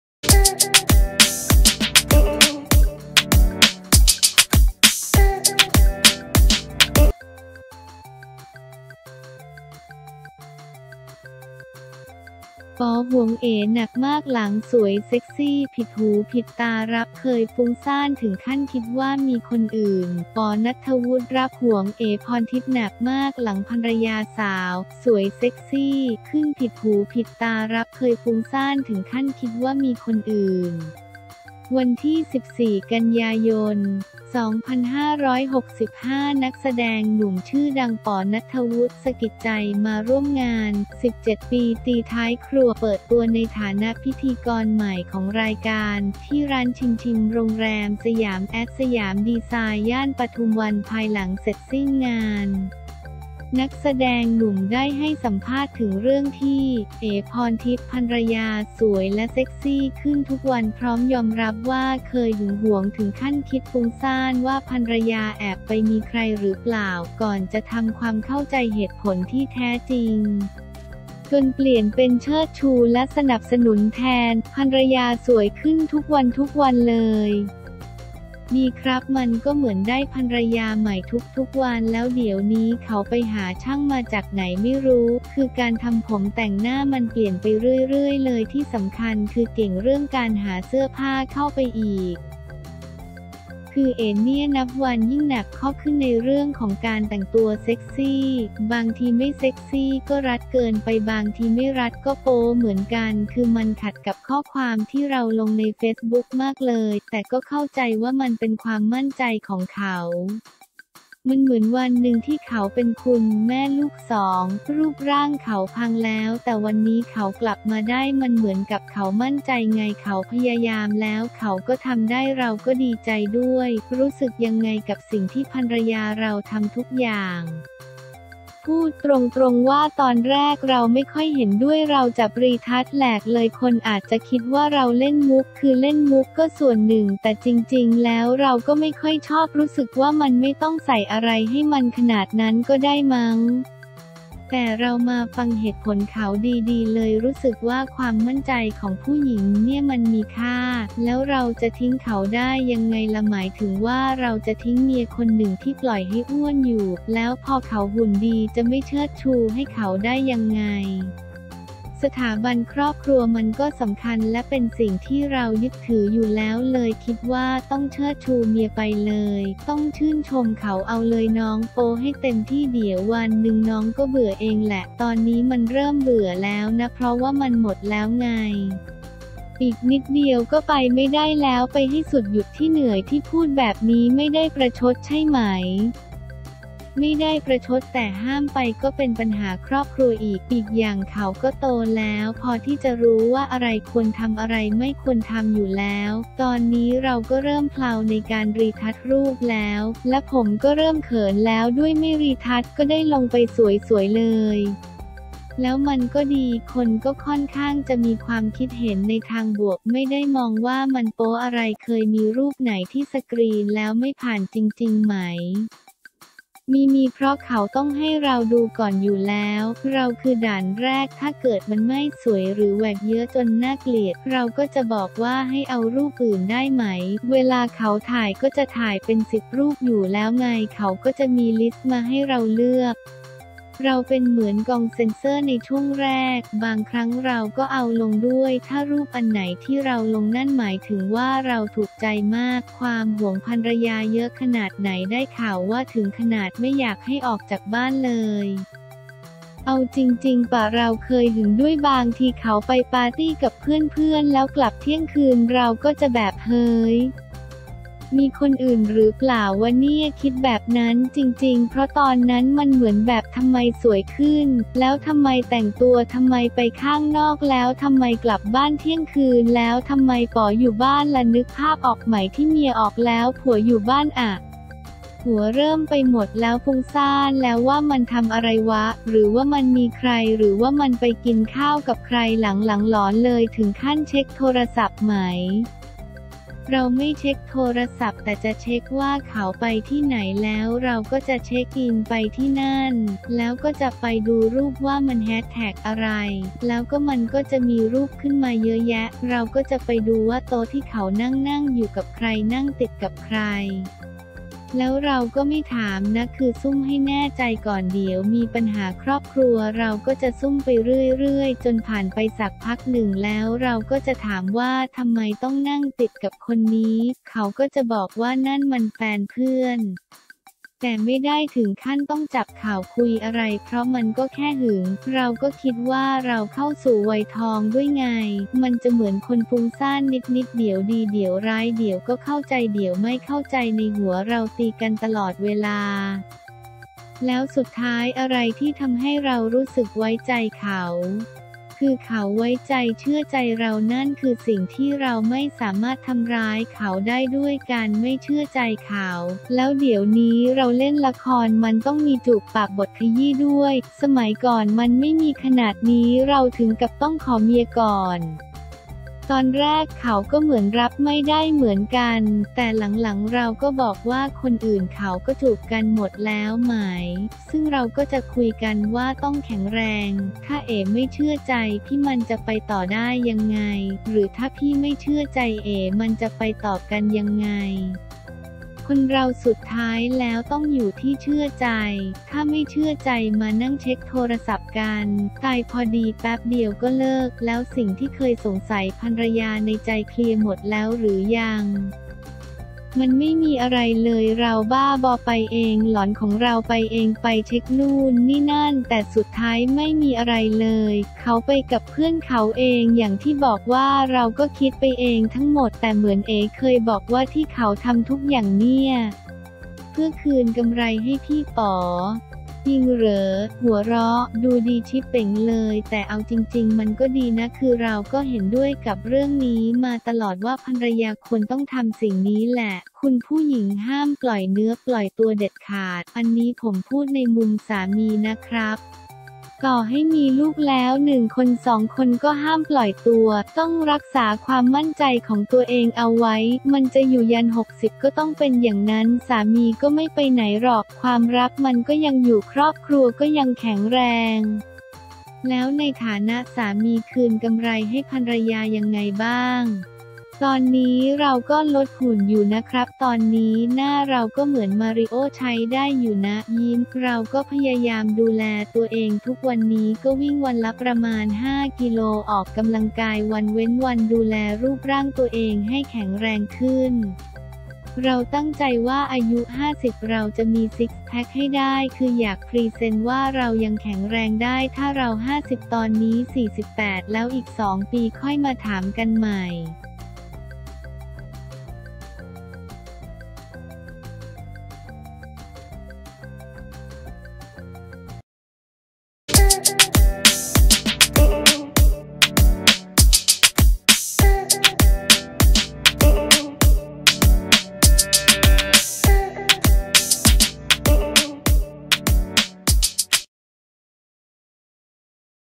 ป๋อ หวง เอ๋ หนักมาก หลัง สวย เซ็กซี่ ผิดหูผิดตา รับเคยฟุ้งซ่านถึงขั้นคิดว่ามีคนอื่น
ป๋อ ณัฐวุฒิ รับหวง เอ๋ พรทิพย์ หนักมาก หลังภรรยาสาว “สวย-เซ็กซี่” ขึ้นผิดหูผิดตา รับเคยฟุ้งซ่านถึงขั้นคิดว่ามีคนอื่น
วันที่ 14 ก.ย. 2565 นักแสดงหนุ่มชื่อดัง ป๋อ ณัฐวุฒิ สกิดใจ มาร่วมงาน “17 ปี ตีท้ายครัว” เปิดตัวในฐานะพิธีกรใหม่ของรายการ ที่ร้าน ChimChim โรงแรม Siam@Siam Design ย่านปทุมวัน ภายหลังเสร็จสิ้นงาน นักแสดงหนุ่มได้ให้สัมภาษณ์ถึงเรื่องที่ เอ๋ พรทิพย์ ภรรยา สวยและเซ็กซี่ขึ้นทุกวัน พร้อมยอมรับว่าเคยหึงหวงถึงขั้นคิดฟุ้งซ่านว่าภรรยาแอบไปมีใครหรือเปล่า ก่อนจะทำความเข้าใจเหตุผลที่แท้จริง จนเปลี่ยนเป็นเชิดชูและสนับสนุนแทน
ภรรยาสวยขึ้นทุกวันๆ เลย? “ดีครับ มันก็เหมือนได้ภรรยาใหม่ทุกๆ วัน แล้วเดี๋ยวนี้เขาไปหาช่างมาจากไหนไม่รู้ คือการทำผมแต่งหน้ามันเปลี่ยนไปเรื่อยๆ เลย ที่สำคัญคือเก่งเรื่องการหาเสื้อผ้าเข้าไปอีก คือเอ๋เนี่ยนับวันยิ่งหนักข้อขึ้นในเรื่องของการแต่งตัวเซ็กซี่ บางทีไม่เซ็กซี่ก็รัดเกินไป บางทีไม่รัดก็โป๊เหมือนกัน คือมันขัดกับข้อความที่เราลงในเฟซบุ๊กมากเลย แต่ก็เข้าใจว่ามันเป็นความมั่นใจของเขา มันเหมือนวันนึงที่เขาเป็นคุณแม่ลูกสอง รูปร่างเขาพังแล้ว แต่วันนี้เขากลับมาได้ มันเหมือนกับเขามั่นใจไง เขาพยายามแล้วเขาก็ทำได้เราก็ดีใจด้วย
รู้สึกยังไงกับสิ่งที่ภรรยาเราทำทุกอย่าง? “พูดตรงๆ ว่าตอนแรกเราไม่ค่อยเห็นด้วย เราจับรีทัชแหลกเลย คนอาจจะคิดว่าเราเล่นมุข คือเล่นมุขก็ส่วนหนึ่งแต่จริงๆ แล้วเราก็ไม่ค่อยชอบ รู้สึกว่ามันไม่ต้องใส่อะไรให้มันขนาดนั้นก็ได้มั้ง แต่เรามาฟังเหตุผลเขาดีๆ เลยรู้สึกว่าความมั่นใจของผู้หญิงเนี่ยมันมีค่า แล้วเราจะทิ้งเขาได้ยังไงล่ะ หมายถึงว่าเราจะทิ้งเมียคนหนึ่งที่ปล่อยให้อ้วนอยู่ แล้วพอเขาหุ่นดีจะไม่เชิดชูให้เขาได้ยังไง สถาบันครอบครัวมันก็สำคัญและเป็นสิ่งที่เรายึดถืออยู่แล้วเลยคิดว่าต้องเชิดชูเมียไปเลย ต้องชื่นชมเขา เอาเลยน้อง โป๊ให้เต็มที่ เดี๋ยววันนึงน้องก็เบื่อเองแหละ ตอนนี้มันเริ่มเบื่อแล้วนะ เพราะว่ามันหมดแล้วไง อีกนิดเดียวก็ไปไม่ได้แล้ว ไปให้สุดหยุดที่เหนื่อย
ที่พูดแบบนี้ไม่ได้ประชดใช่ไหม? “ไม่ได้ประชด แต่ห้ามไปก็เป็นปัญหาครอบครัวอีก อีกอย่างเขาก็โตแล้วพอที่จะรู้ว่าอะไรควรทำอะไรไม่ควรทำอยู่แล้ว ตอนนี้เราก็เริ่มเพลาในการรีทัชรูปแล้ว และผมก็เริ่มเขินแล้วด้วย ไม่รีทัชก็ได้ ลงไปสวยๆ เลย แล้วมันก็ดี คนก็ค่อนข้างจะมีความคิดเห็นในทางบวก ไม่ได้มองว่ามันโป๊อะไร